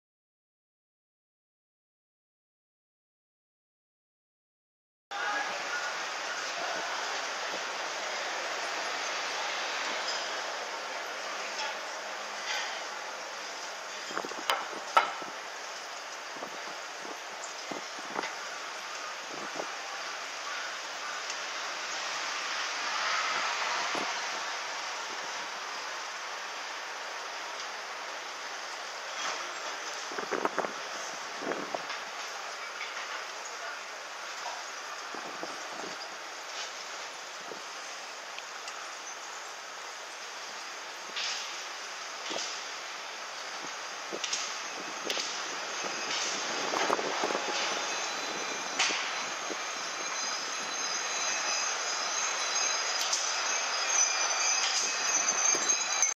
雷門スタート、裏道サイクリング
雷門横のホテル。そこからサイクリング。
裏道で団子屋まで